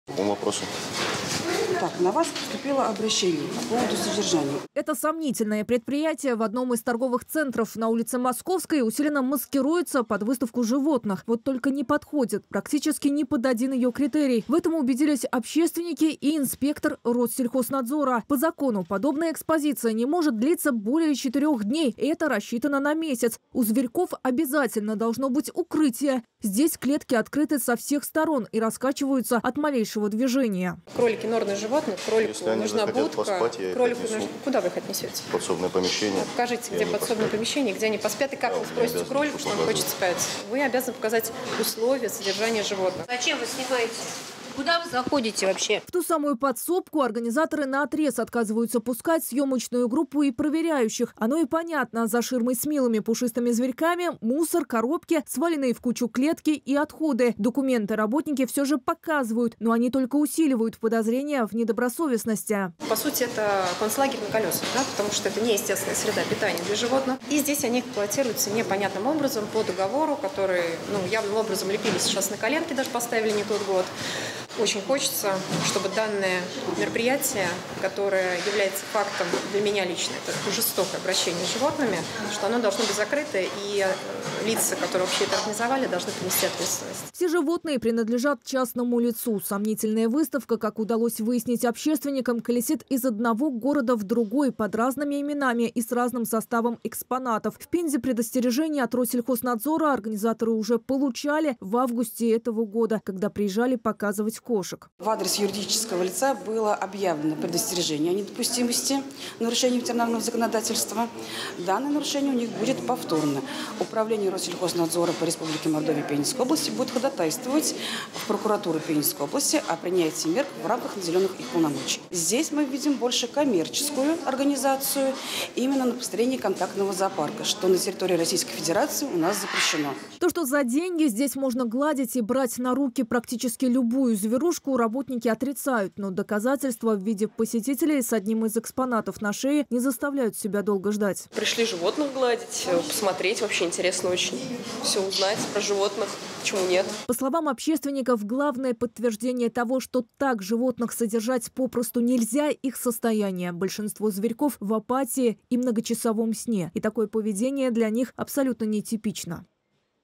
Внимание общественников привлекла выставка животных, которая открылась в одном из торговых центров
Сомнительное предприятие в одном из торговых центров на улице Московской усиленно маскируется под выставку животных. Вот только не подходит практически ни под один критерий. Так, по закону она не может длиться более четырех дней, эта рассчитана на месяц. У зверьков обязательно должно быть укрытие, здесь клетки открыты со всех сторон и раскачиваются от малейшего движения. Все подробности – в видеоматериале.